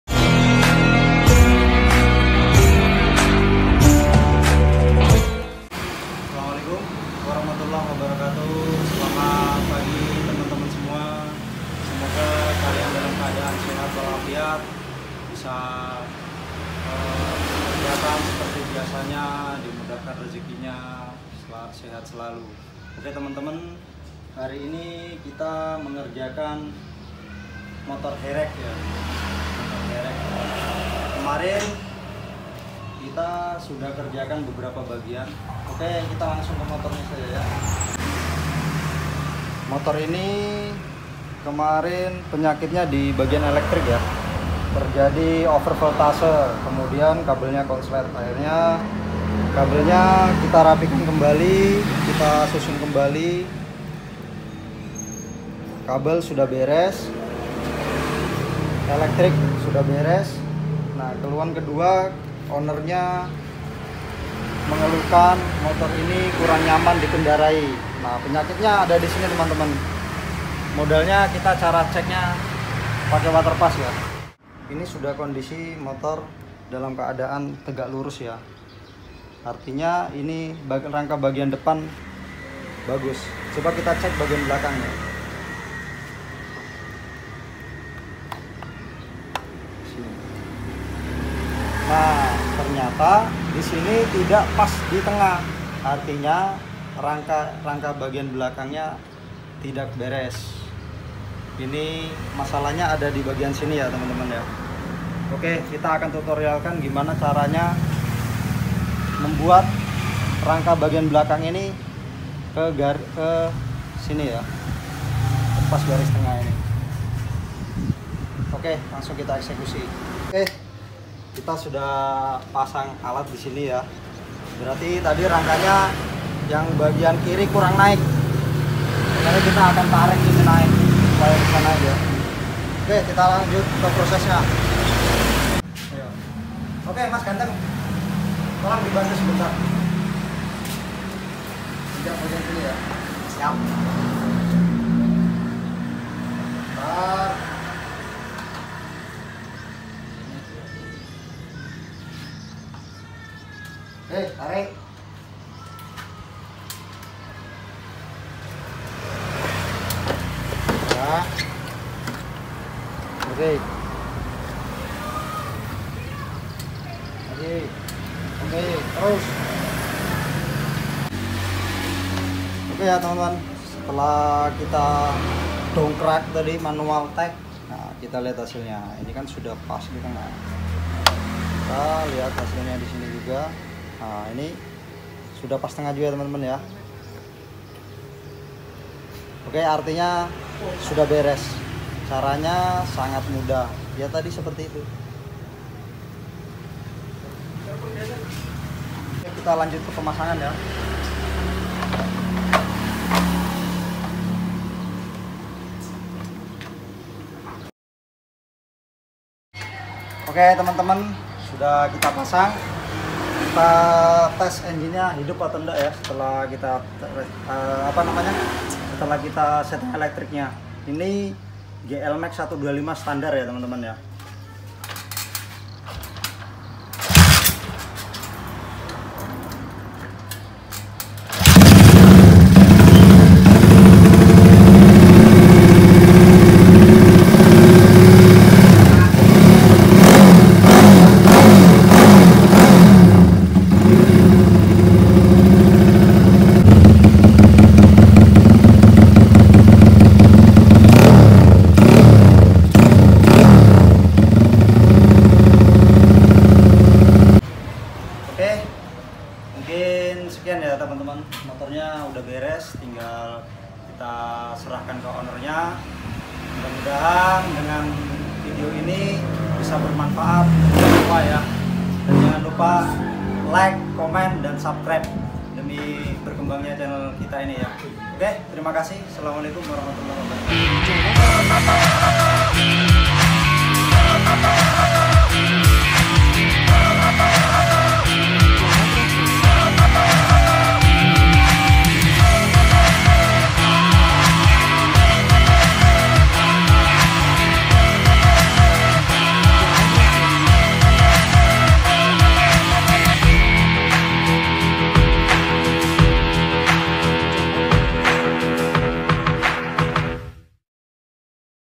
Mega pro mati total? Ini penyebabnya!!
Penataan ulang kabel body motor megapro dan tata ulang rangka yang tidak sejajar pada motor megapro